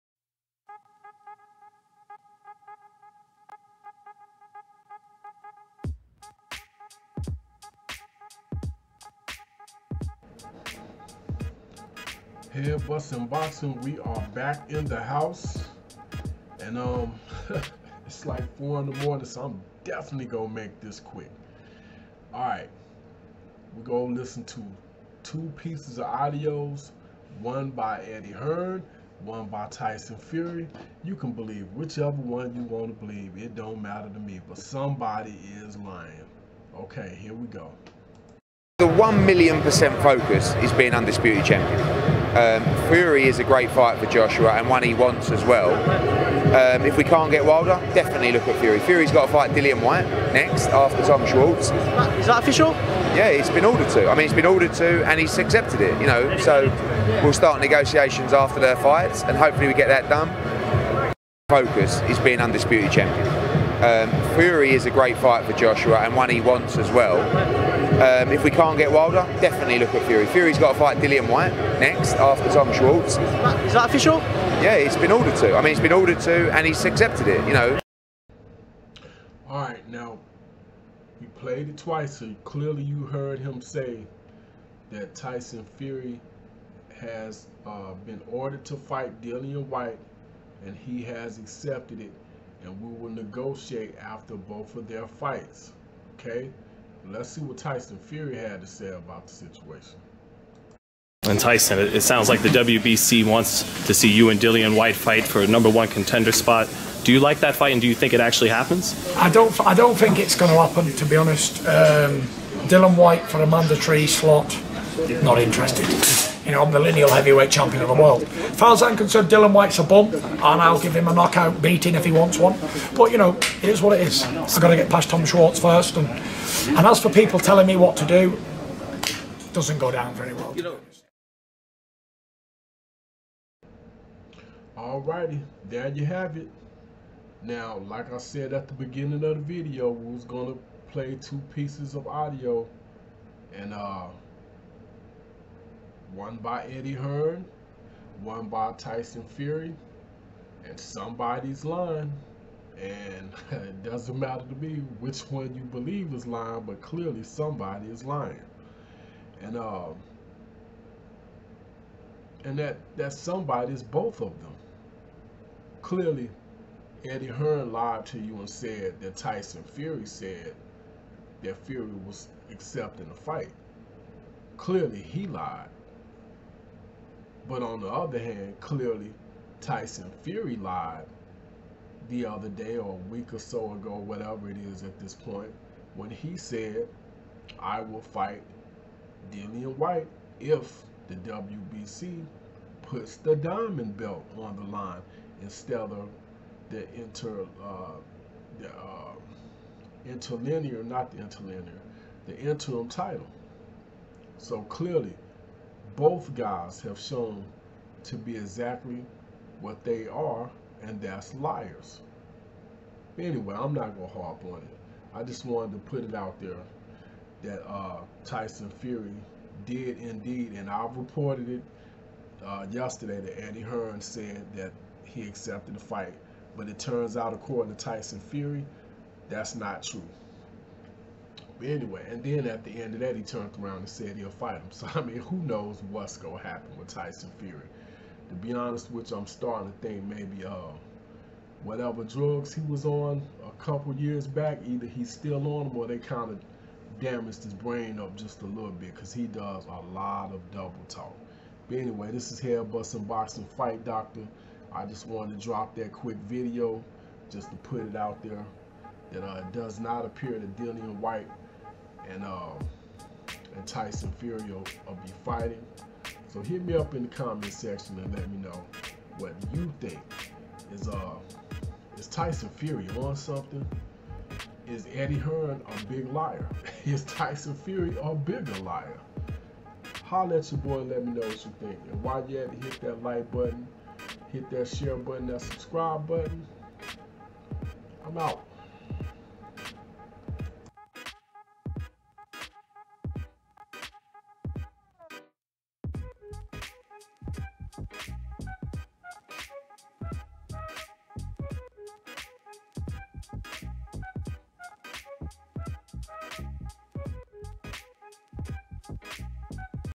FURY SAYS HE ISN'T INTERESTED IN FIGHT DILLIAN WHYTE FOR THE WBC DIAMOND BELT
ACCORDING TO EDDIE HEARN TYSON FURY AGREED TO FIGHT DILLIAN WHYTE FOR THE WBC DIAMOND BELT. WELL TYSON FURY CAME OUT AND SAID HE ISN'T INTERESTED IN FIGHTING DILLIAN WHYTE, AND TELLING HIM WHAT TO DO DON'T WORK SO WELL.